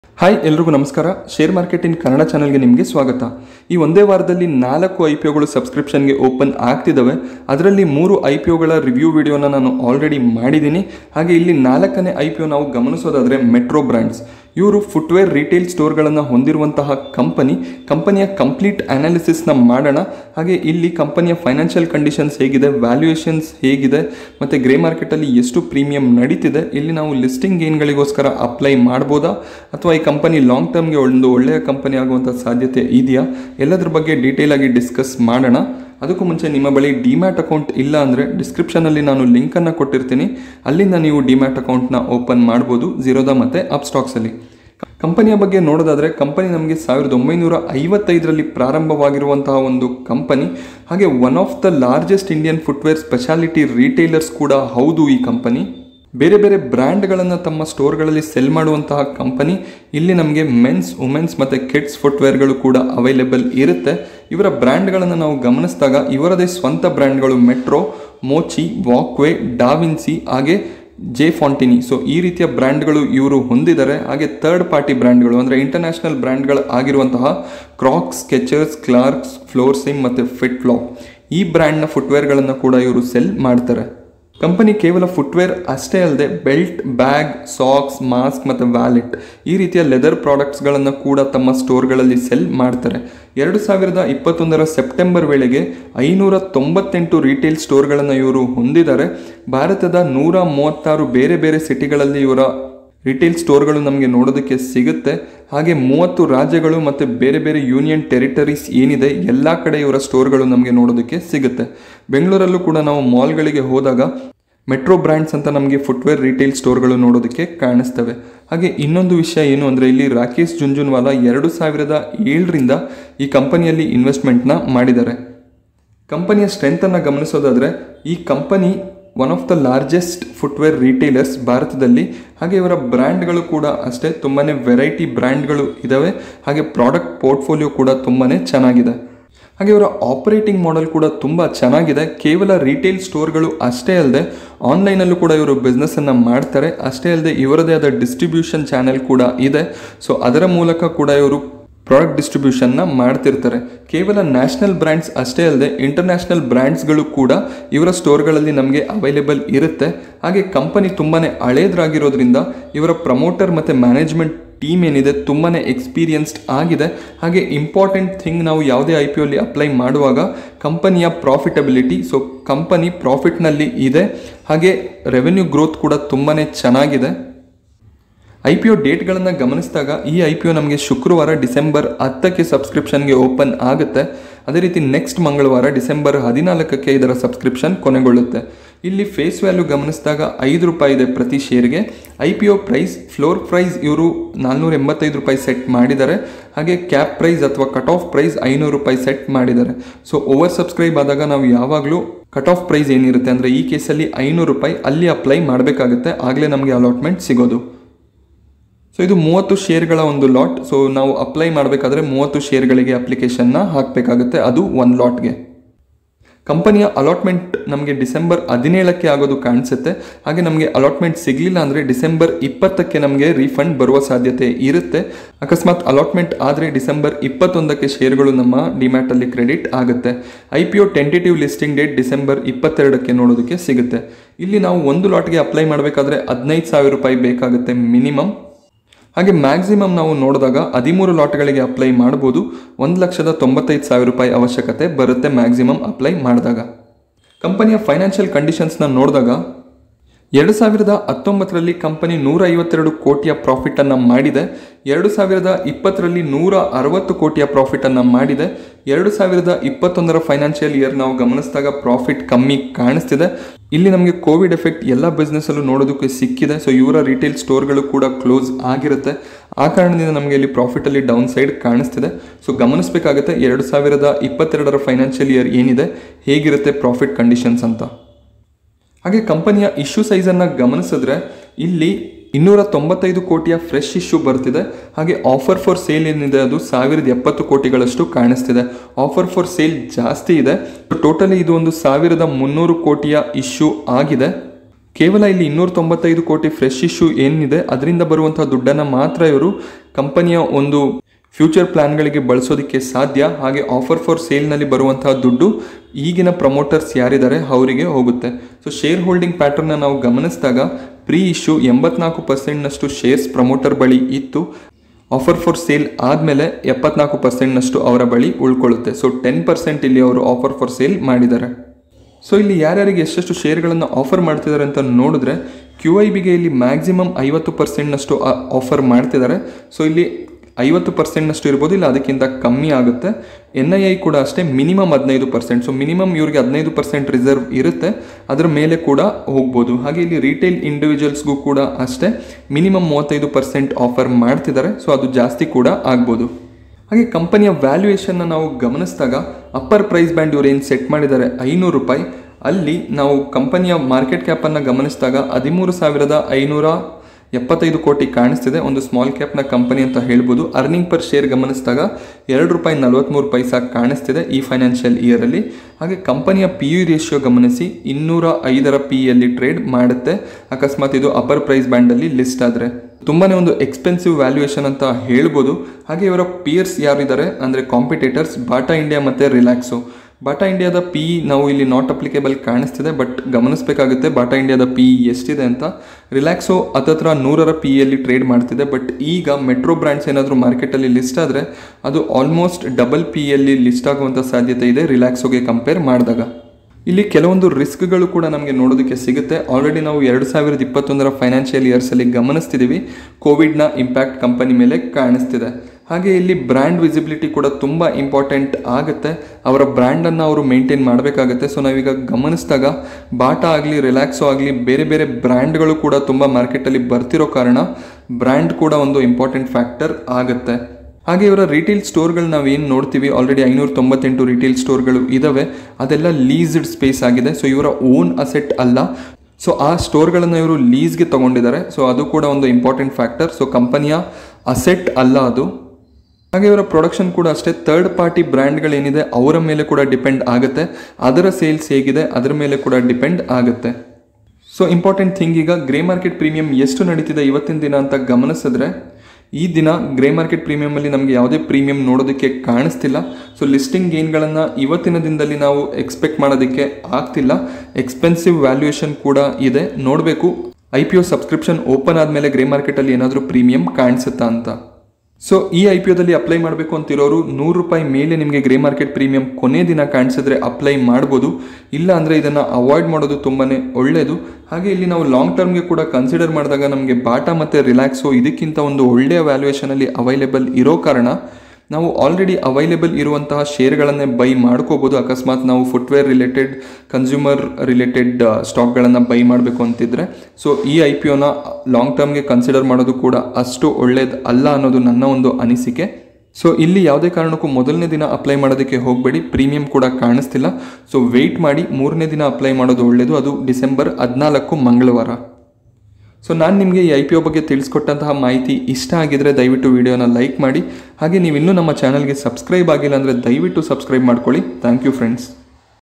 Metro Brands IPO review in Kannada| Metro Brands IPO dates and GMP| Apply or Avoid
Metro Brands in a Famous Foot ware brand in India having a good number of retail stores coming up with an IPO of 1300 crores. Find out complete details about IPO in the Video.

Metro Brands IPO Review in Kannada
Metro Brands IPO Details

Link To open free demat account in Upstox